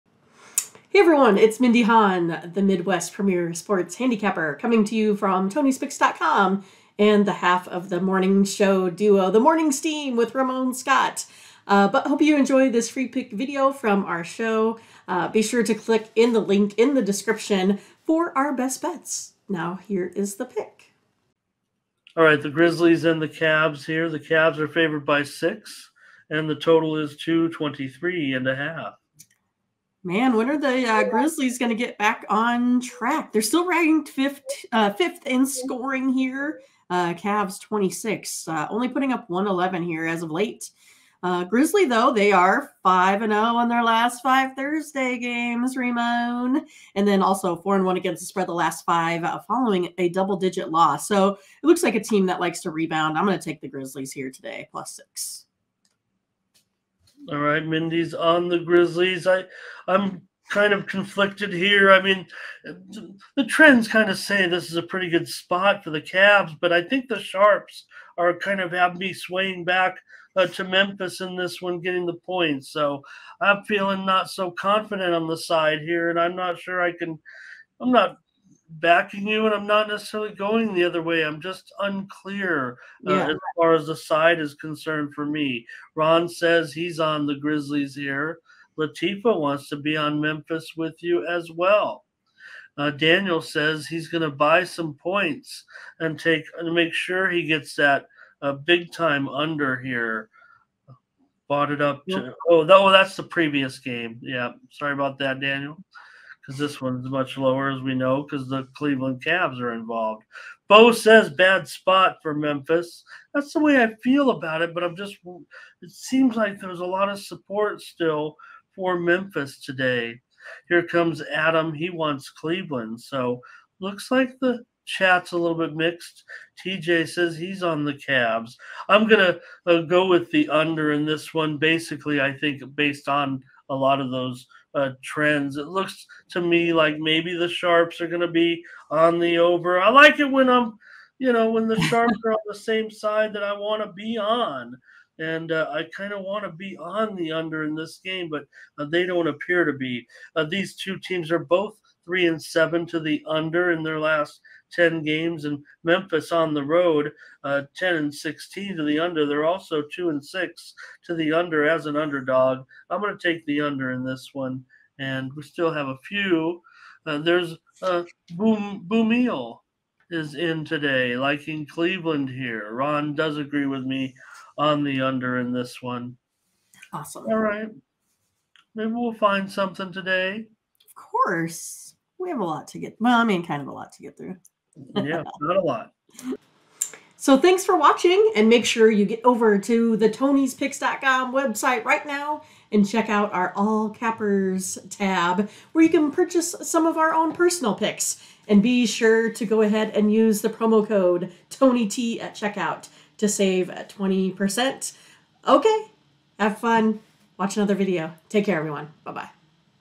Memphis Grizzlies vs Cleveland Cavaliers 2/2/2023 FREE NBA Game Analysis on Morning Steam Show
2/2/2023 Memphis Grizzlies vs Cleveland Cavaliers FREE NBA Game Analysis on Morning Steam Show